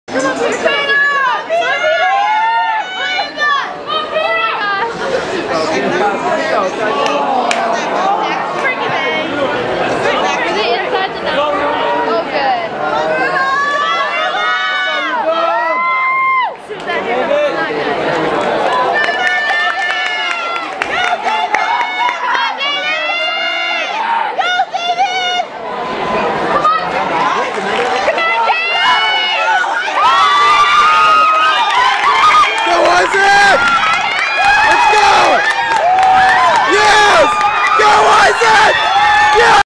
Isaac Wicks 4x100m Prelim State Championships
Isaac is running anchor.  He had to play catch up in this race just to qualify for finals.